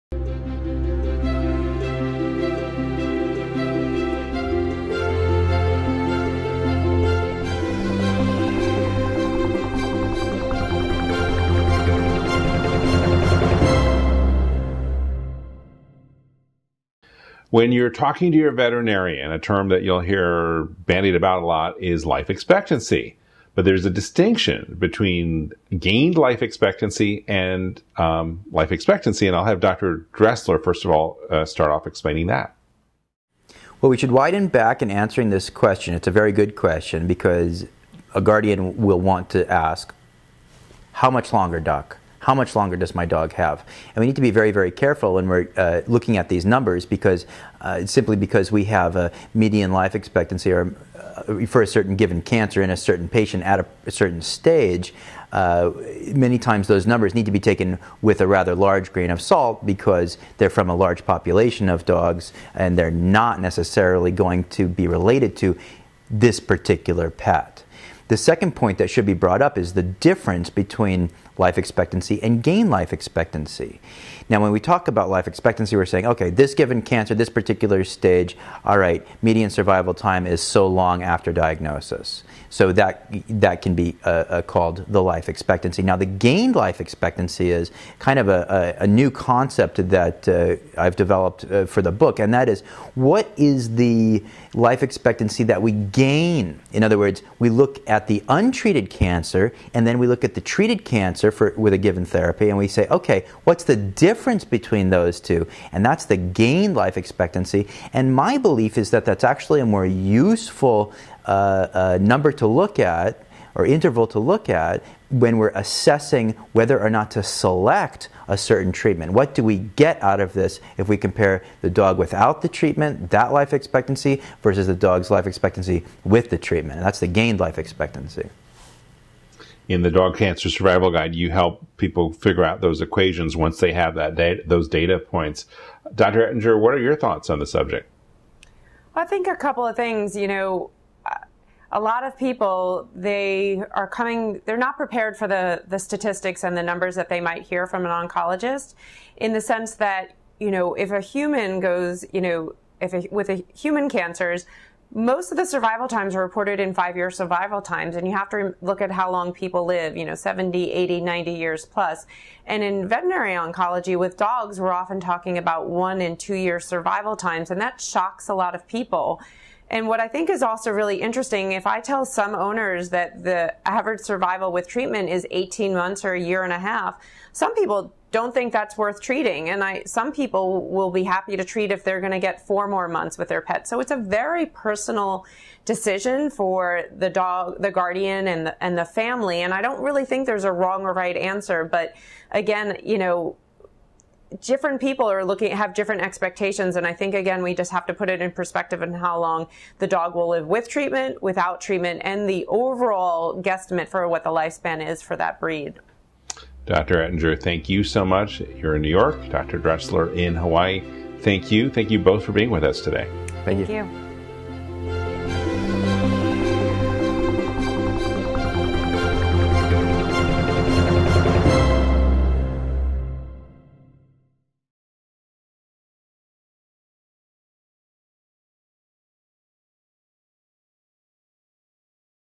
Gained Life Expectancy Vs. Life Expectancy
Dr. Dressler and Dr. Ettinger, co-authors of “The Dog Cancer Survival Guide” discuss life expectancy following diagnosis from a new angle.

Topics Covered:
The difference between Life Expectancy and Gained Life Expectancy
How to use Life and Expectancy and Gained Life Expectancy in Treatment Plan Analysis
Statistics and Survival Times

Get to know today's guests:
Sue Ettinger, DVM, Dip. ACVIM (Oncology):